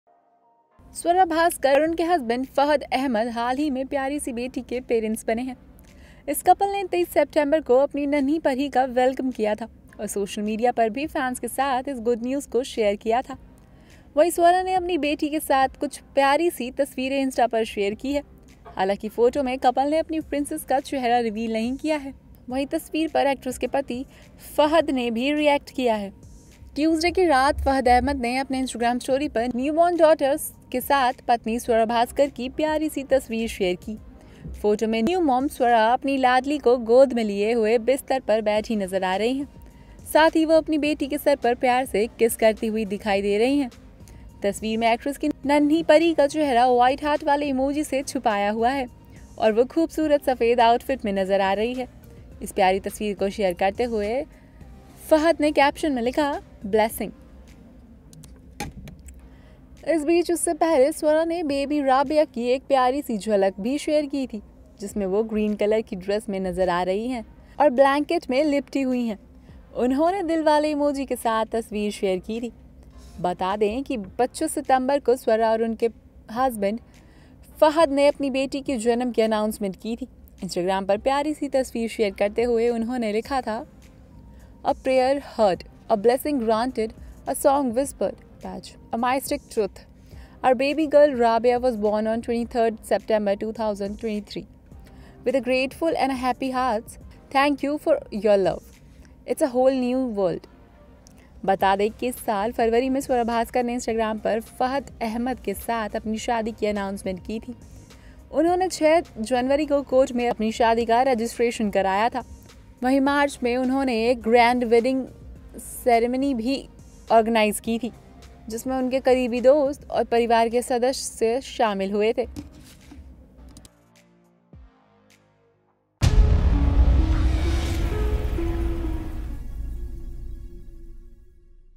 न्यू बॉर्न बेटी राबिया पर प्यार लुटाती नज़र आईँ Swara Bhaskar
स्वरा भास्कर और उनके पति फहद अपनी लाइफ के सबसे खूबसूरत फेज में हैं. कपल ने हाल ही में बेटी का वेलकम किया है. वहीं स्वरा ने अब अपनी बेटी संग प्यारी तस्वीर शेयर की है.

Swara Bhaskar and her husband Fahadh are in the most beautiful phase of their life. The couple has recently welcomed a daughter. Swara has now shared a lovely picture with her daughter.